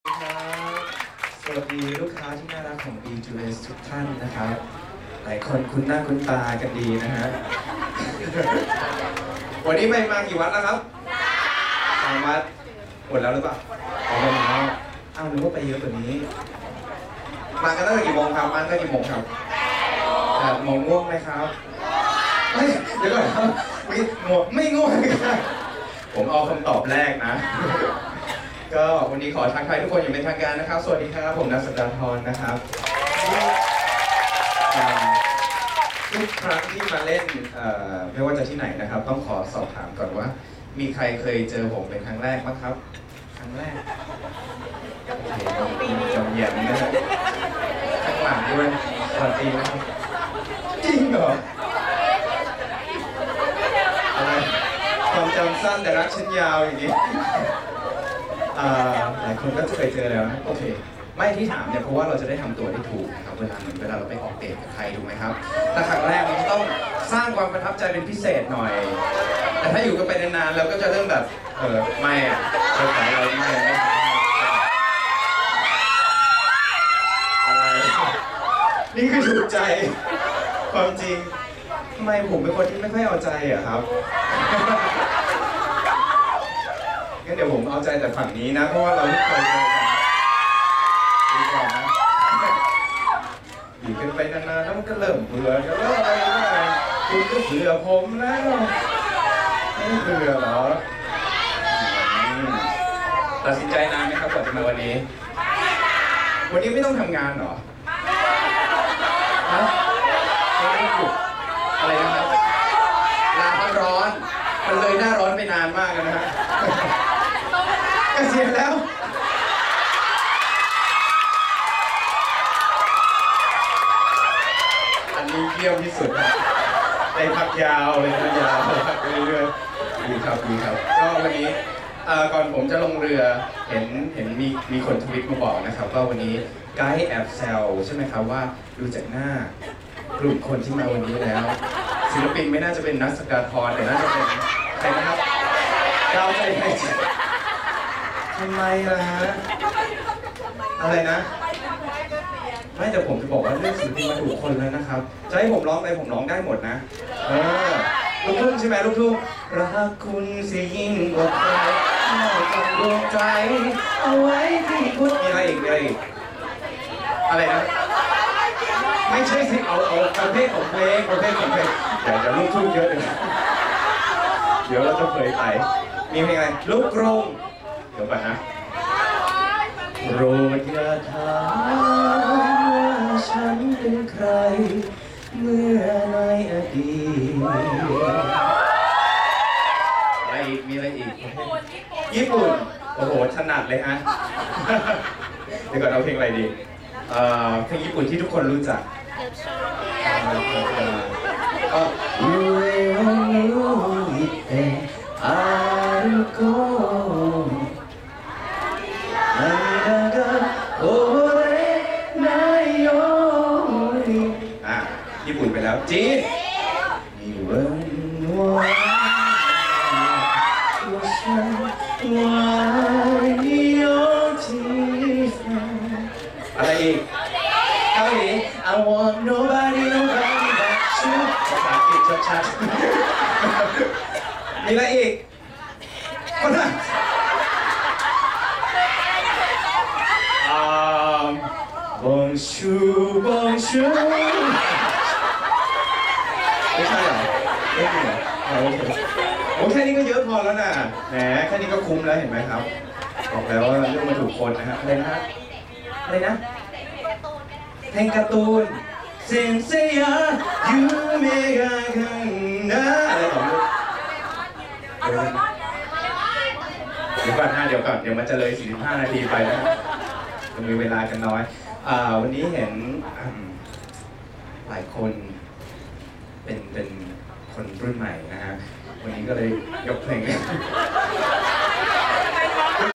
18.12.13 ร้องเพลงหลายๆแนว (ล่องเรือไหว้พระกับซุปตาร์ 12ปีB2S) - ณัฐ ศักดาทร
18.12.13 ณัฐ ศักดาทร พูดคุย และร้องเพลงหลายๆแนวให้ได้ฟังแบบสั้นๆ ในงาน "ล่องเรือไหว้พระกับ ซุปตาร์ ในโอกาสครบรอบ 12 ปี B2S"   "B2S I'm 12th Anniversary" โดย A-time Traveller